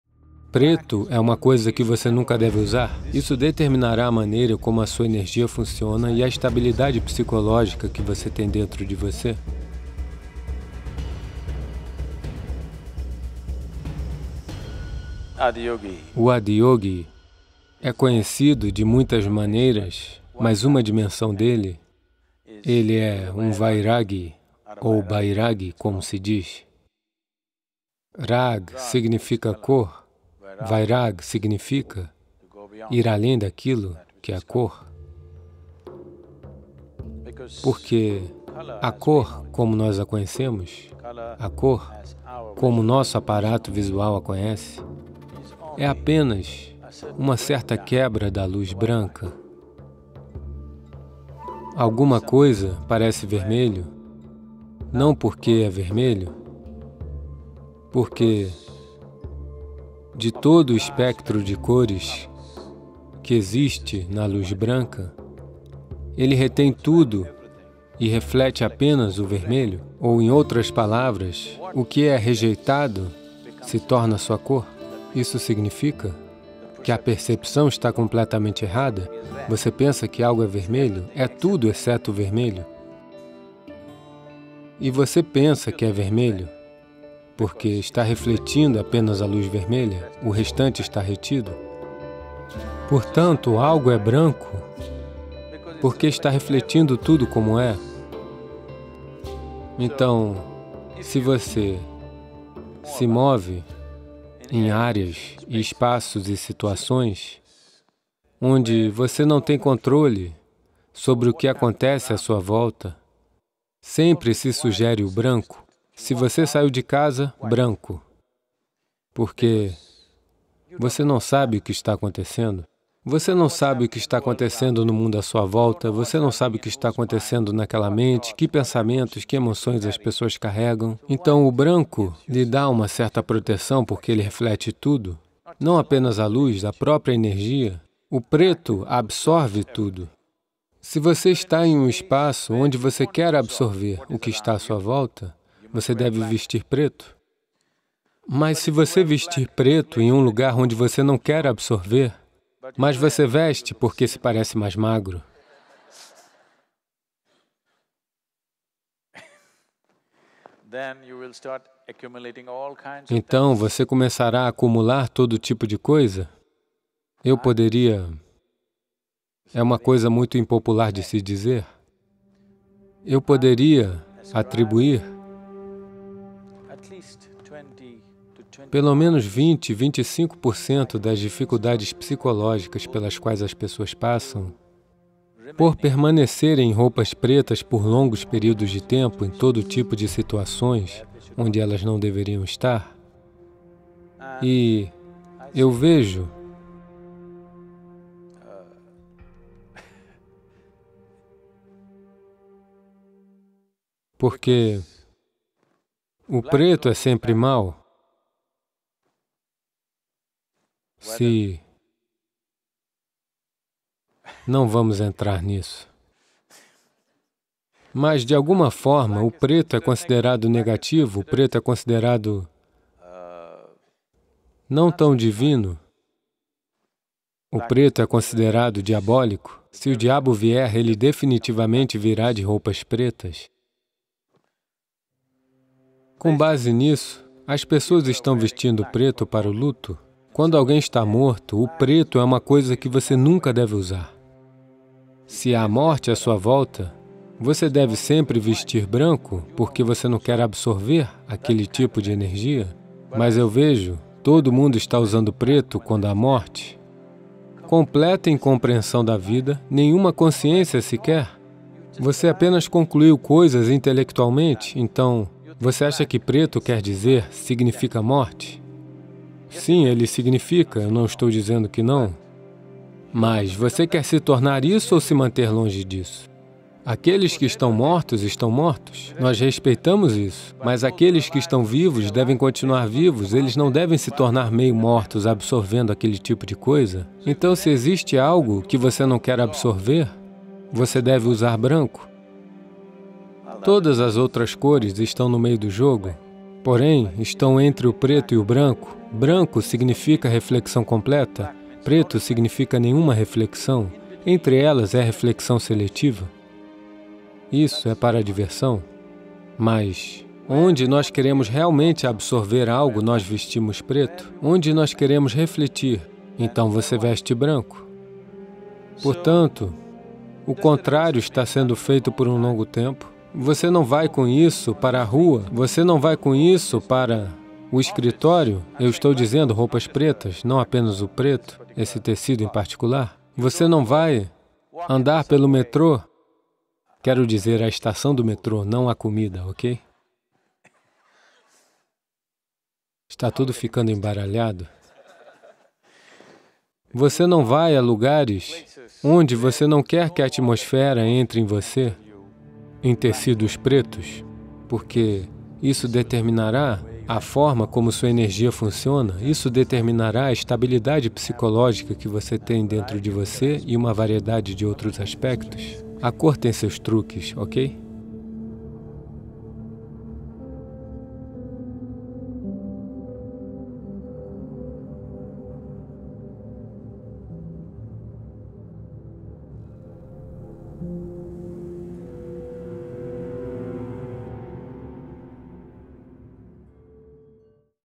Pense Duas Vezes Antes de Usar Roupas Pretas | Sadhguru Português
Os estilistas adoram o preto, mas o preto vem com suas armadilhas. Sadhguru fala sobre o significado e os perigos do uso do preto e nos diz quando devemos absolutamente evitá-lo. Este vídeo é um trecho da consagração de Adi Yogi - The Abode of Yoga (A Morado do Yoga), no Isha Institute of Inner-sciences, EUA.